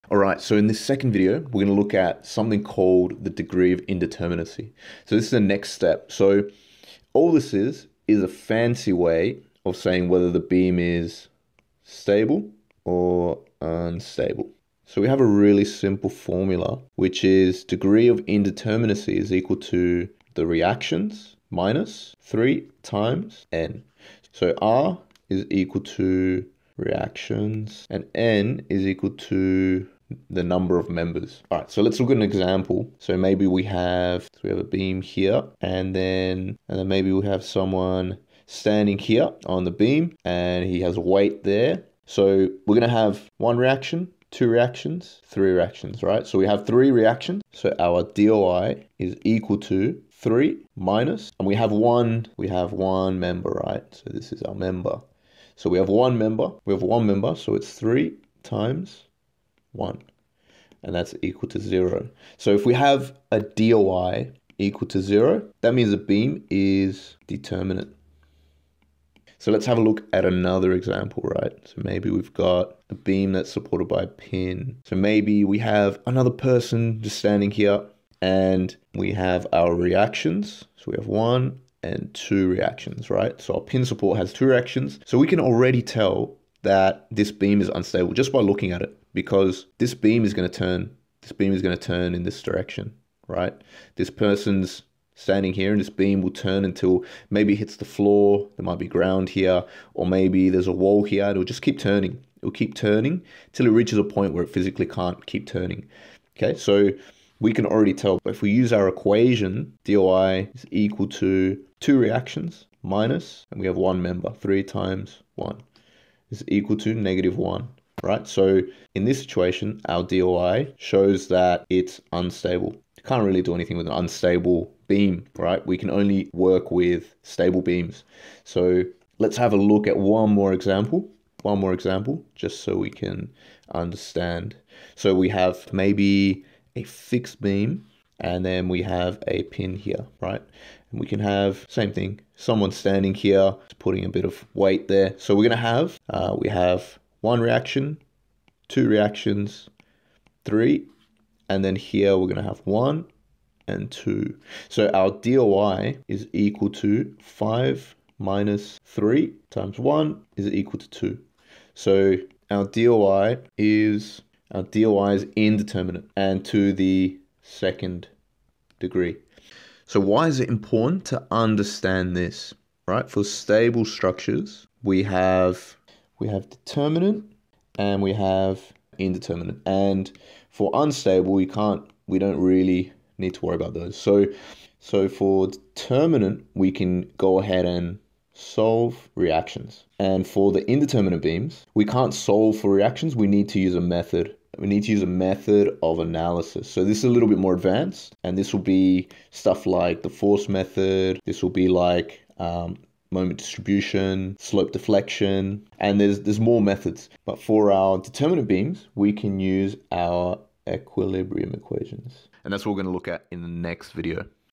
Beams Intro 2. Degree of Indeterminacy (DOI)
This video is on the concept of Degree of indeterminacy of a simple beam. This is the second video in the beam analysis series.